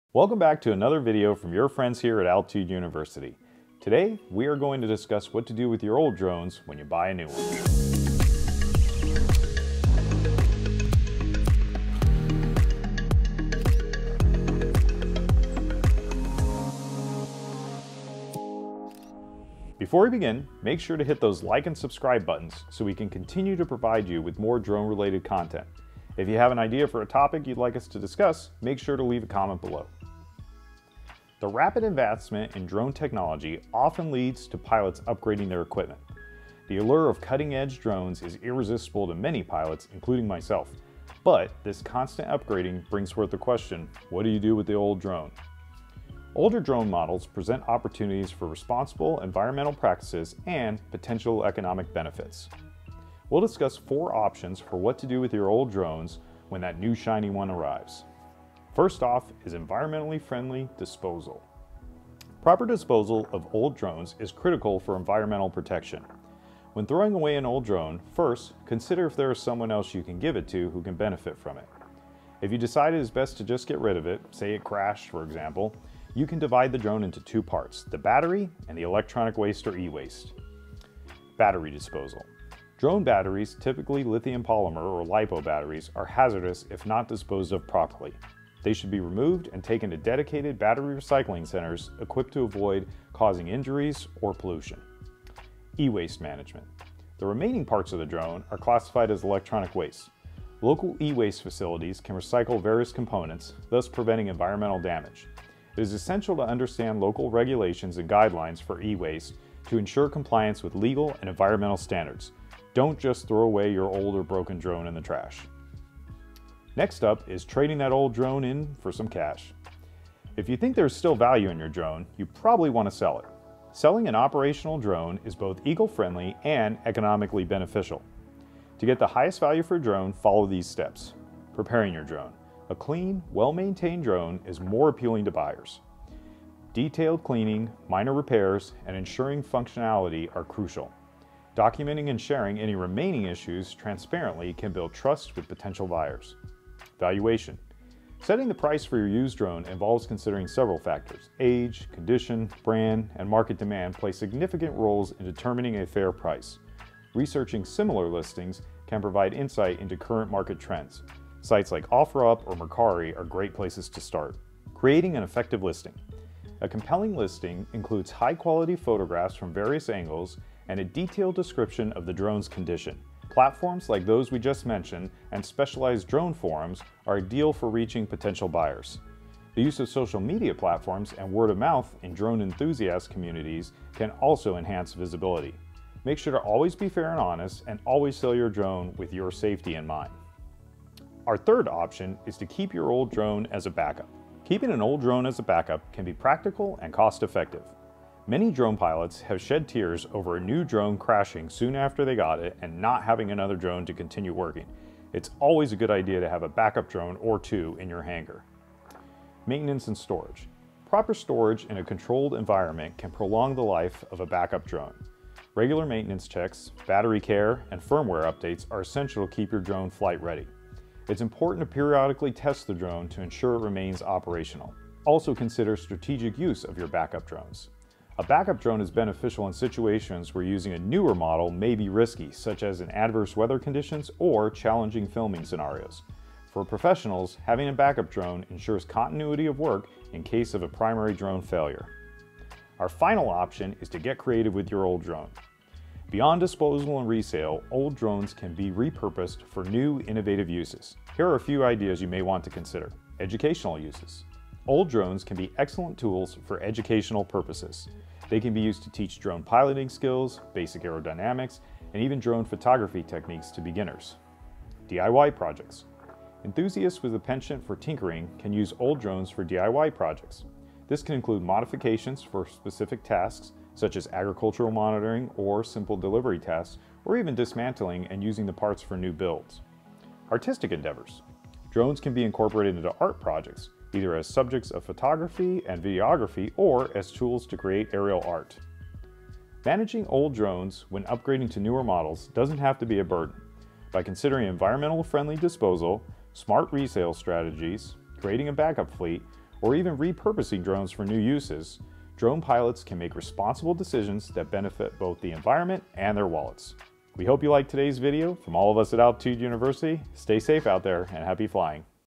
Unlock the Hidden Value of Old Drones
What do you do with old drones when your new drone arrives. Checkout out our ideas from selling your drone for some cash to educating other on the benefits of UAV technology!